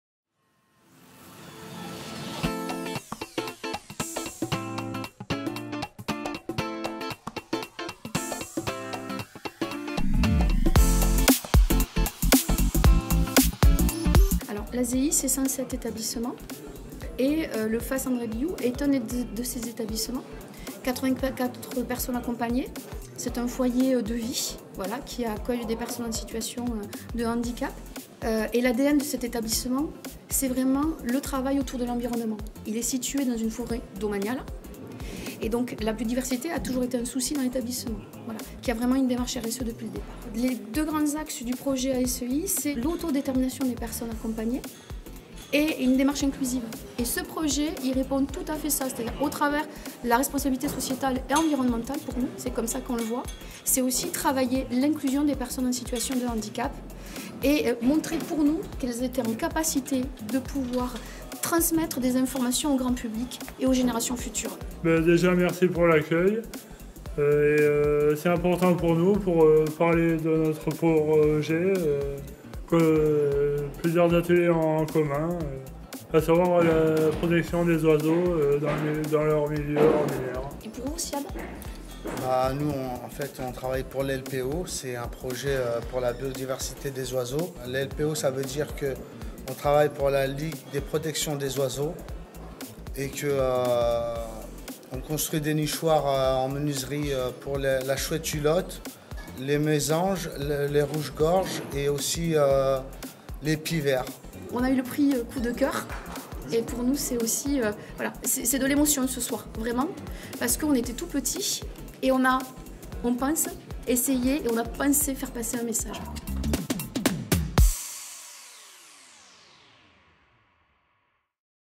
ASEI : Nuit de la RSE 2019, Lauréat Trophée Coup de Cœur, “Meilleure démarche collaborative”
Projet : Handicap & Environnement : le pouvoir d’agir ensemble !

ASEI est une association qui accompagne et forme des personnes en situations de handicap et des personnes dépendantes et fragilisées. L’association s’est investie dans la vie de la commune et a pour ambition de sensibiliser les résidents, écoles et touristes à la protection de l’environnement. Pour cela, elle s’associe à la Ligue pour la Protection des Oiseaux.

Pour vous faire découvrir ce projet, Marie-Laure MANHES, Directrice adjointe des établissements et services du secteurs médico-social, Léo TURCO et Cyad GEMMA, résidents du Foyer André Billoux et membres de la LPO.

Pour en savoir plus sur l’ASEI et ses projets, rendez-vous